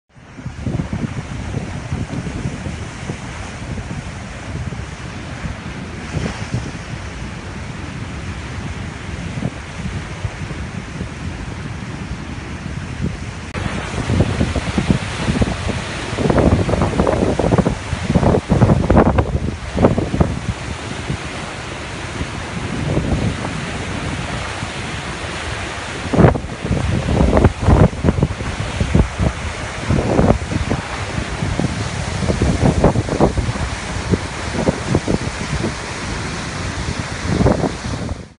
繼富岡紅燈塔倒塌 綠島紅燈塔也在半空搖搖欲墜
莫蘭蒂颱風吹毀台東市富岡港紅燈塔，近中午綠島在地居民也發現綠島南寮漁港港嘴的紅燈塔也傾斜在半空中。